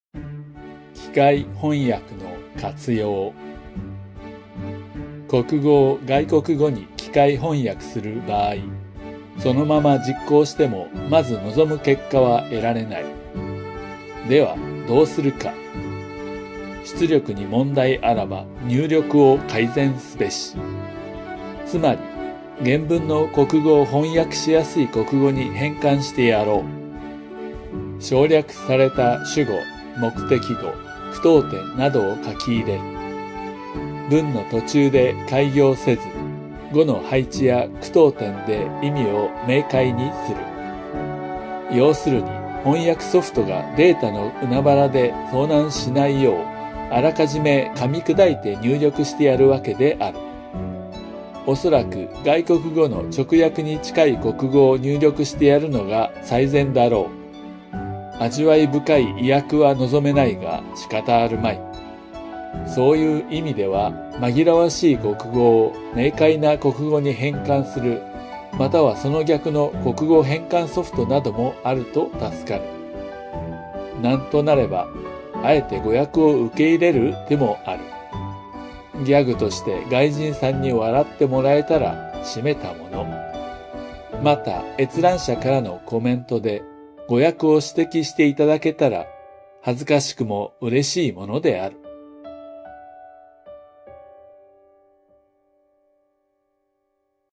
Tome文芸館 機械翻訳の活用
声の出演･音響･原作･映像：Tome館長　自作自演。

国語を外国語に機械翻訳する場合
そのまま実行しても、まず望む結果は得られない。

では、どうするか？

出力に問題あらば、入力を改善すべし。
つまり、原文の国語を翻訳しやすい国語に変換してやろう。

省略された主語、目的語、句読点などを書き入れる。
文の途中で改行せず、語の配置や句読点で意味を明解にする。

要するに、翻訳ソフトがデータの海原で遭難しないよう
あらかじめ噛み砕いて入力してやるわけである。

おそらく外国語の直訳に近い国語を入力してやるのが最善だろう。
味わい深い意訳は望めないが、仕方あるまい。

そういう意味では、紛らわしい国語を明解な国語に変換する
または、その逆の国語変換ソフトなどもあると助かる。

なんとなれば、あえて誤訳を受け入れる手もある。
ギャグとして外人さんに笑ってもらえたら、しめたもの。

また、閲覧者からのコメントで、誤訳を指摘していただけたら
恥ずかしくも嬉しいものである。

[ Utilizing Machine Translation ]

When translating a national language machine
into a foreign language, even if it is executed as it is,
the desired result can not be obtained first.

Then what should we do?

If there is a problem with the output,
you should improve the input.

In other words, you translate the original language
into a language that is easy to translate.

Enter the omitted subjects, objects,
punctuation marks and so on.

Do not break at the middle of a sentence,
make clear the meaning at word placement and punctuation.

In short, I will chew and input beforehand so that
translation software will not be disturbed at the sea of ​​data.

Perhaps it would be best to enter a language
close to the literal translation of a foreign language.

I can not hope for a tasty translation, but it can not be helped.

In that sense,
it is helpful to convert misleading national languages
​​into clear national languages, or vice versa.

In any case, there are also dare to accept mistranslation.
If a foreigner laughs as a gag, it is what you stopped.

Also, if you can point out the mistranslation with comments
from the viewer, it would be embarrassing but also glad.